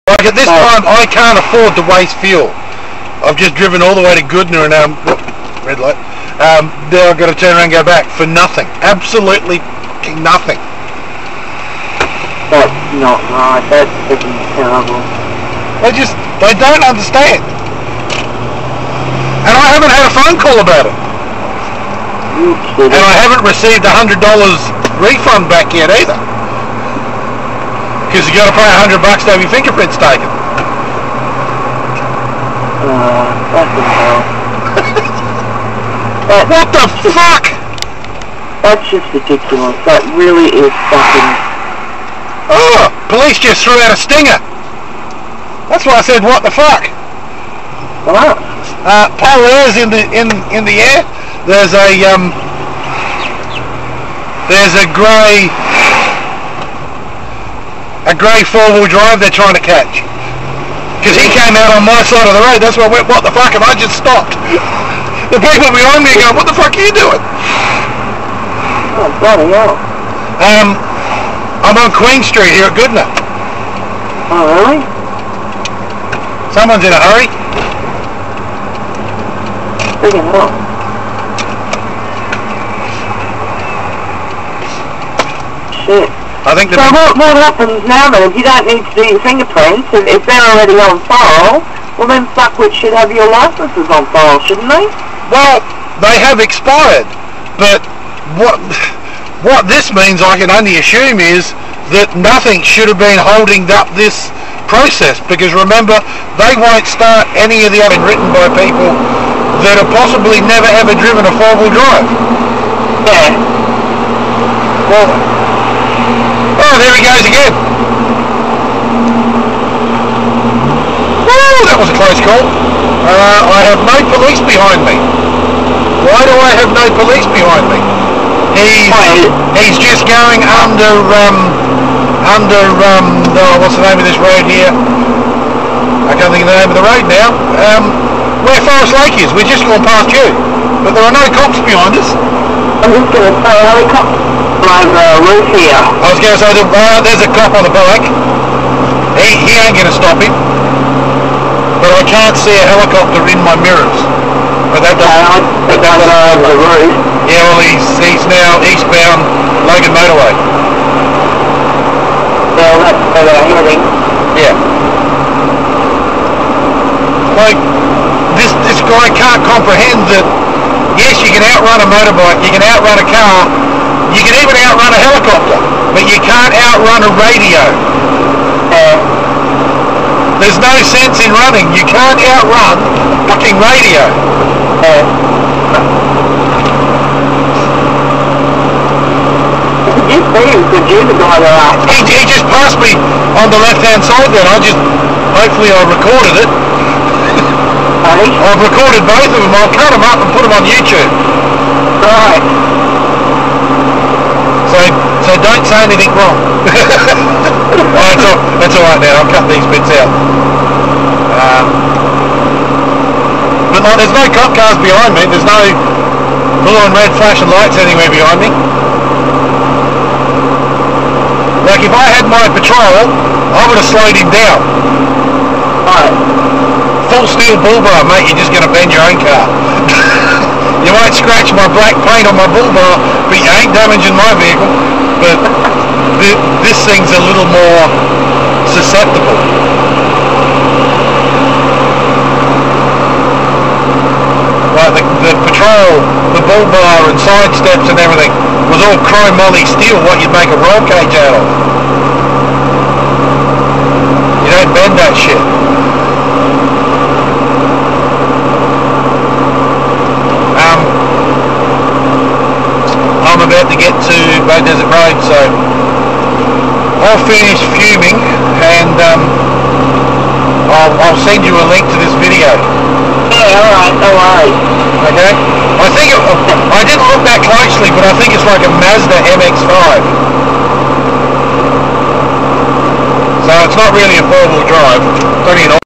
Police Chase Stinger deployment Goodna 05-07-2016
I am VERY sorry for all of the swearing in the back ground.
I am talking to a college on the phone (via Bluetooth) about a government departments stuff ups.

This was filmed on my Dashcam in 720p